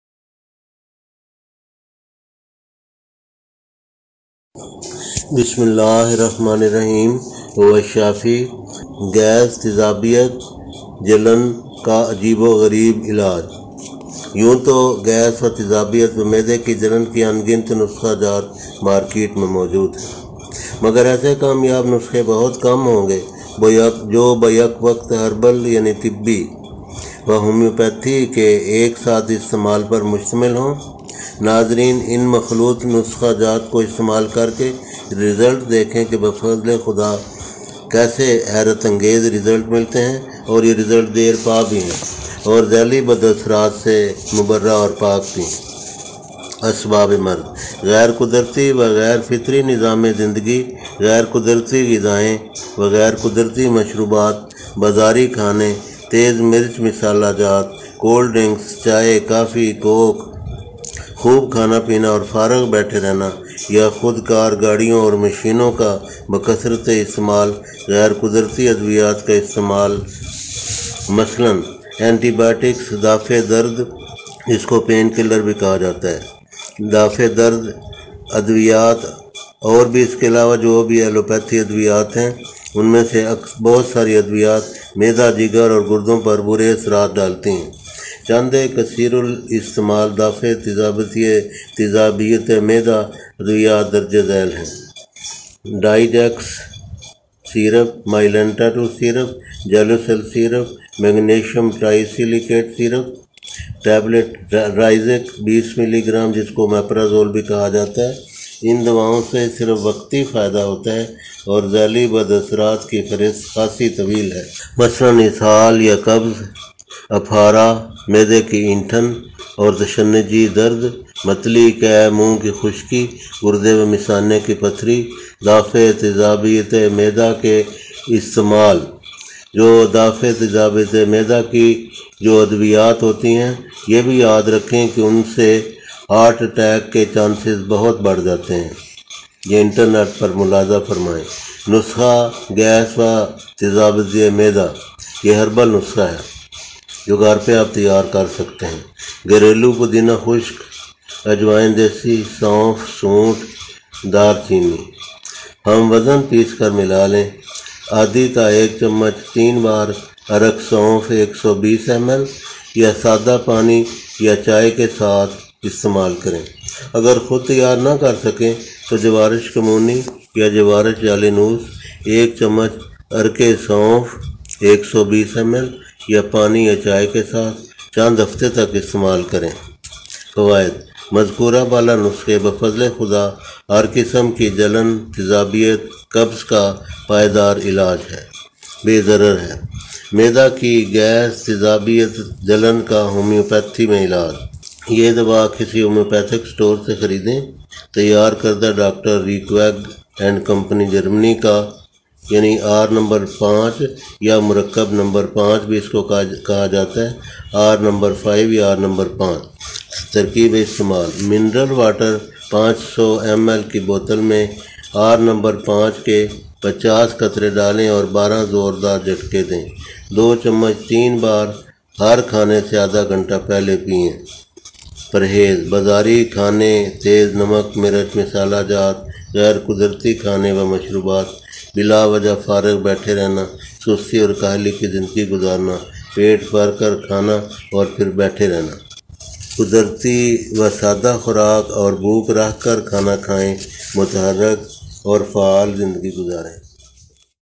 Gas tezabeyat acidity ka asal herbal homeo elaj by Dr Mazhar Easy Health: no negative side effects
We provide here details of Gas tezabeyat acidity ka asal herbal homeo elaj by Dr Mazhar Easy Health: no negative side effects.
تیزابیت اور پیٹ کی گیس کا ہربل ہومیو آزمودہ دیسی علاج، بغیر منفی اثرات کے۔

Dr Mazhar
# Herbal